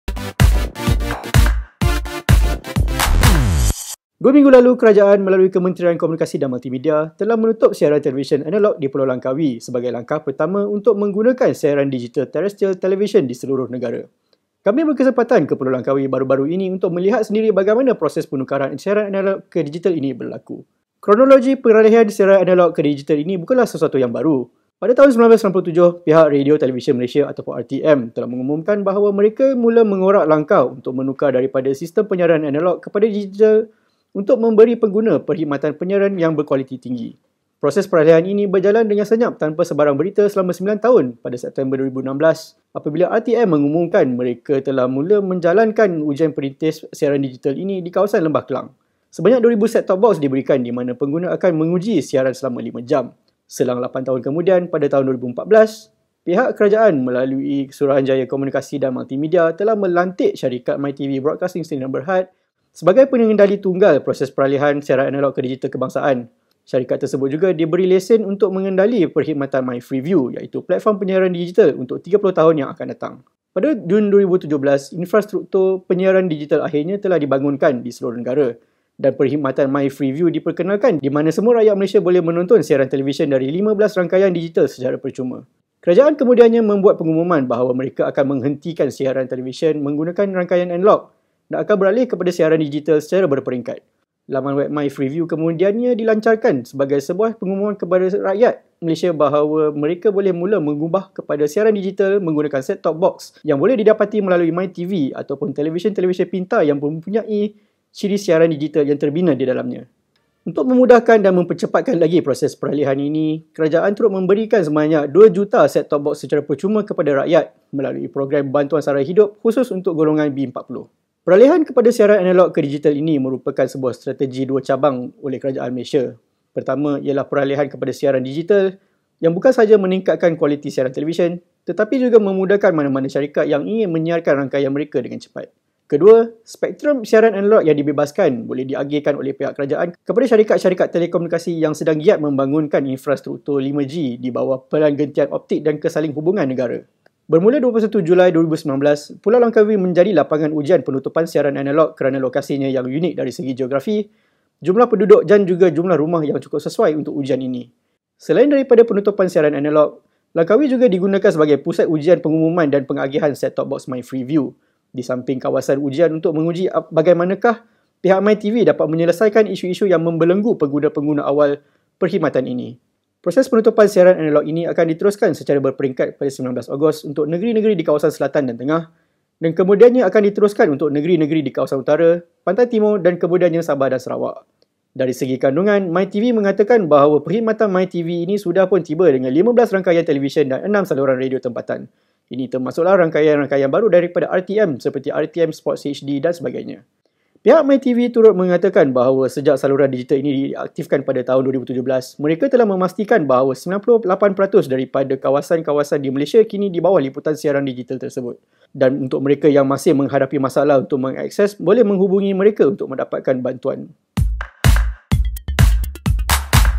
Siaran TV Analog Akan Ditamatkan Pada 30 September Ini
Kerajaan telah pun memulakan peralihan siaran televisyen secara analog ke digital secara berperingkat, dan kini mereka berkongsi lanjut, mengumumkan siaran televisyen analog akan ditamatkan sepenuhnya pada 30 September 2019 ini. Sebagai langkah persediaan, peralihan ke siaran tv digital telah pun dilakukan, dan pada video kali ini kami melihat secara ringkas berkenaan dengan peralihan ini.